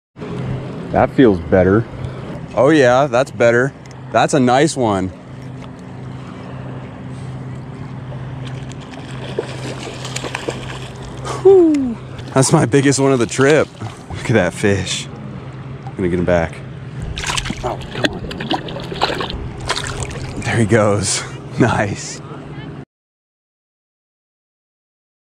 TROUT Fishing for BIG Fish!! (Wallowa Lake)
In today's short, we go TROUT Fishing for BIG Fish at Wallowa Lake in Northeastern Oregon! :)